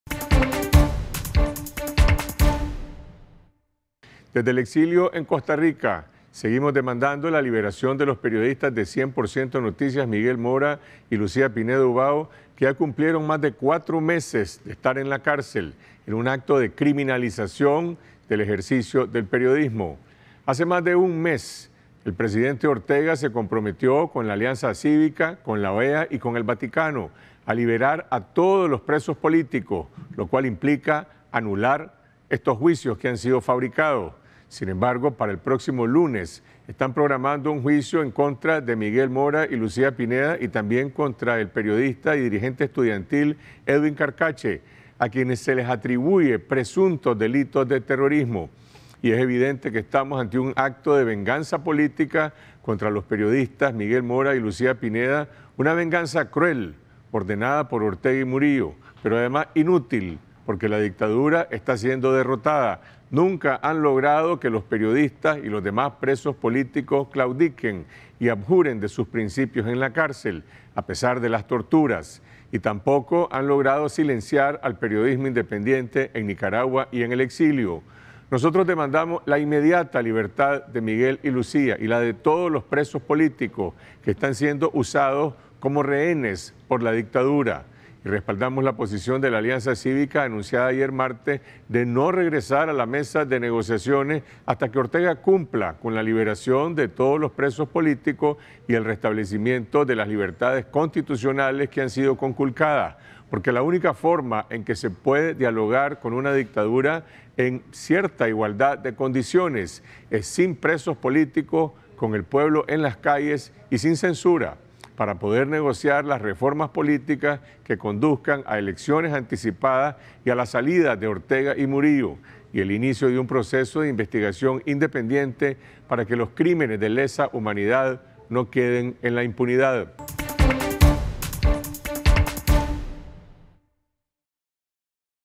Carlos F. Chamorro: libertad para Miguel Mora y Lucia Pineda
El comentario de Carlos F. Chamorro.